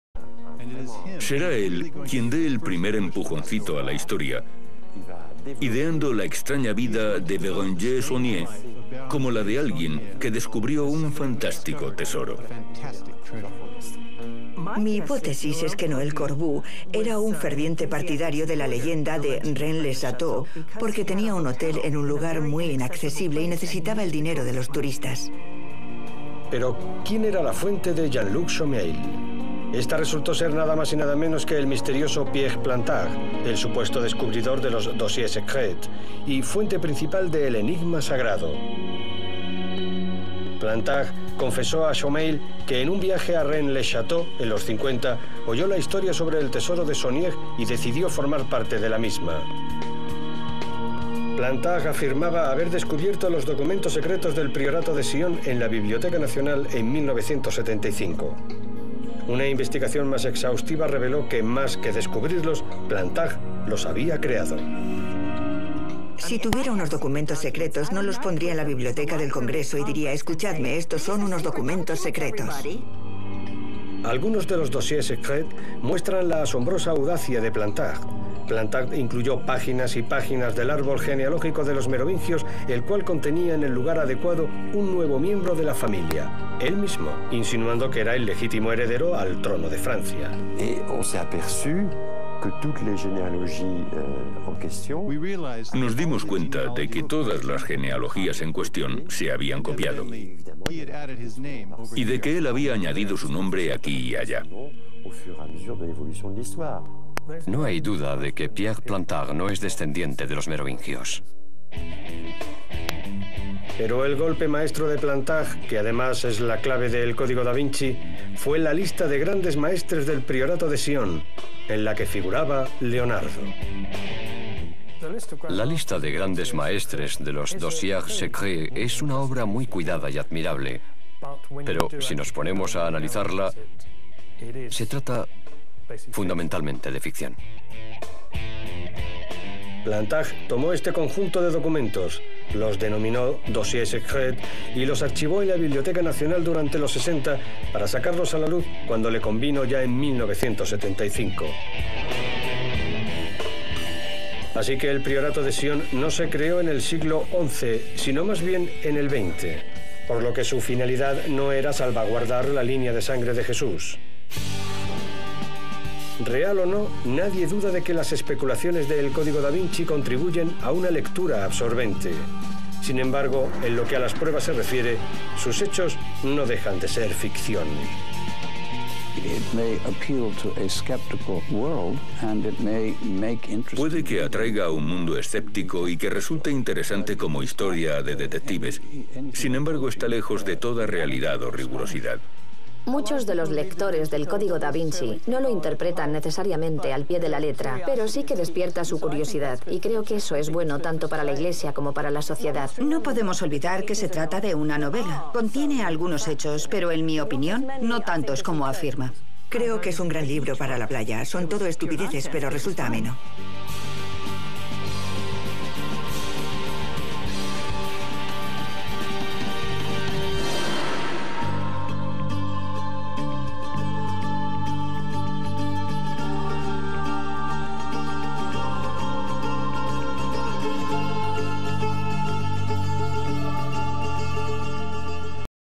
EL CODIGO DA VINCI (REVELACIONES 3)
De Paris hasta Londres, pasando por Hollywood, el Código da Vinci está por todos lados. Pareciera que todo el mundo lo ha leído. ¿Por qué es tan popular este libro? ¿Es pura ficción o revela una verdad oculta? Si las teorías conspiratorias de "El Código Da Vinci" son ciertas. ¿Seria posible que el linaje de Jesús y María Magdalena siguiera existiendo en la actualidad?